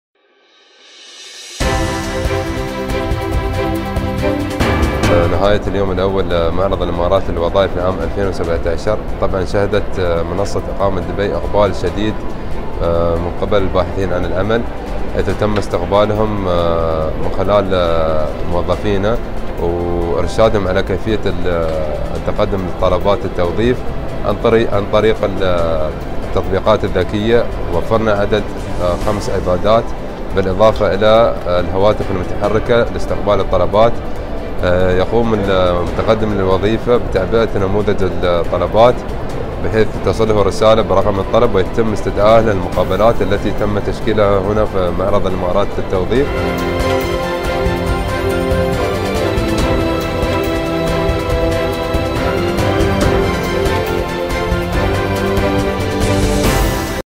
مقابلة النقيب احمد المري في معرض الإمارات للوظائف2017
مقابلة النقيب احمد المري مدير إدارة الموارد البشرية بإقامة دبي في معرض الإمارات للوظائف2017